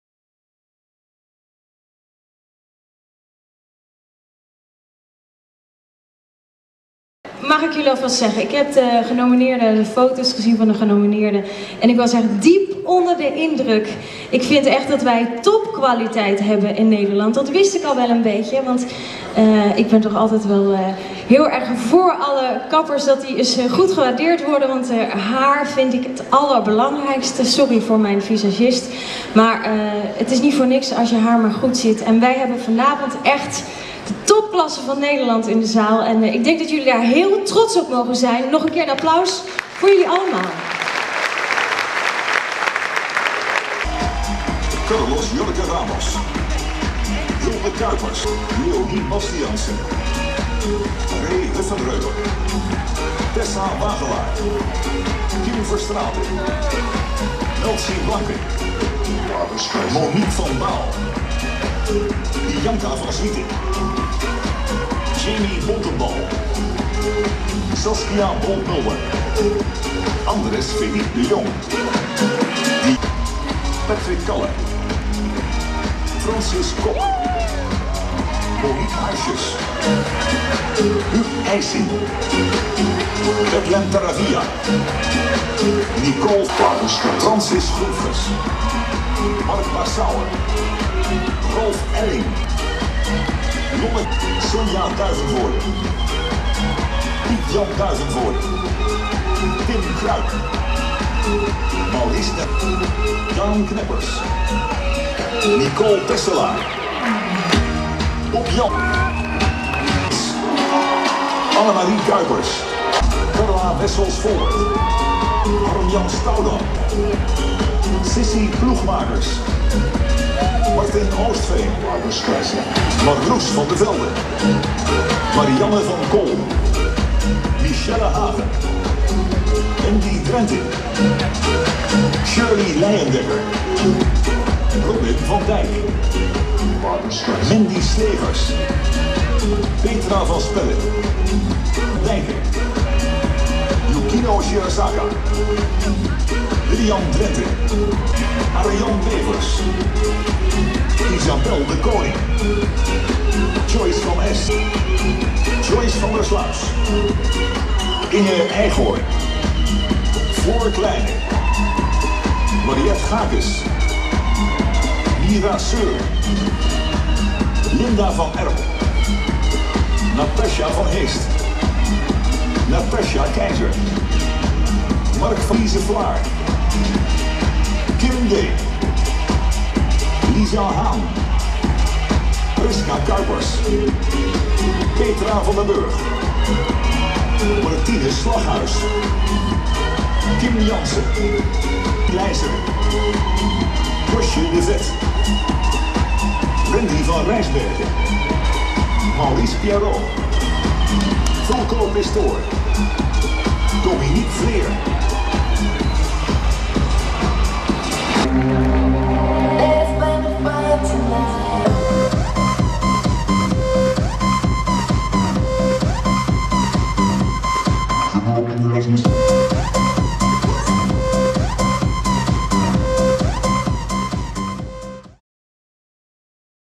Alle genomineerden voor de Coiffure Award 2012
Opkomst van alle genomineerden voor de Coiffure Award 2012 tijdens de gala-avond van de coiffure awards 2012. Presentatie Chantal Janzen.